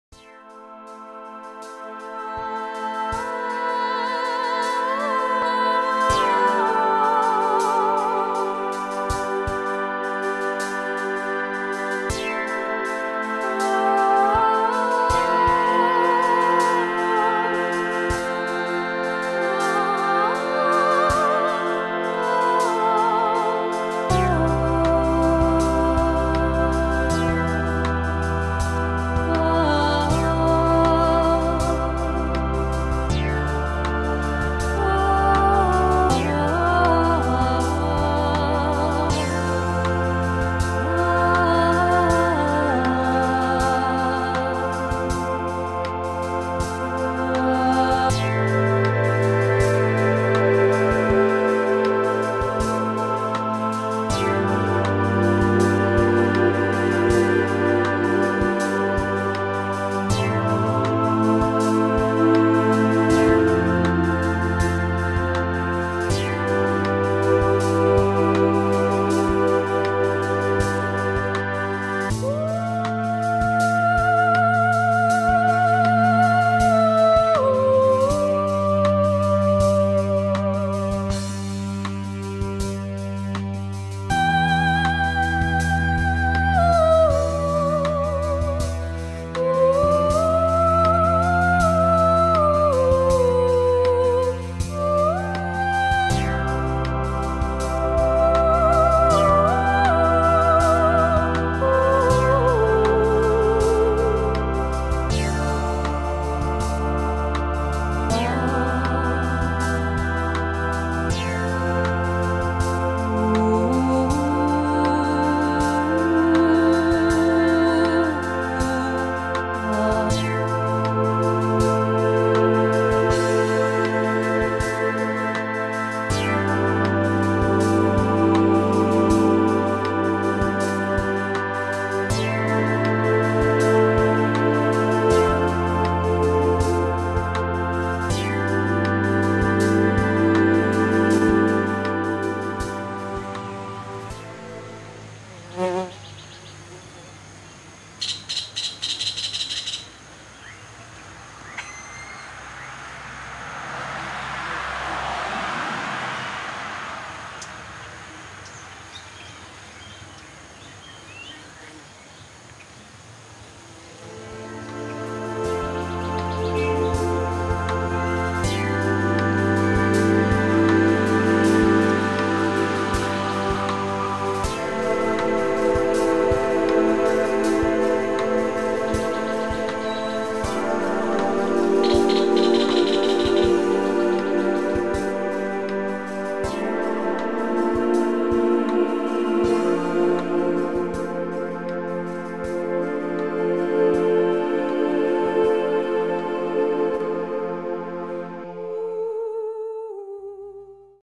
Instantes Naturales
Video que grabé en la huerta de papá.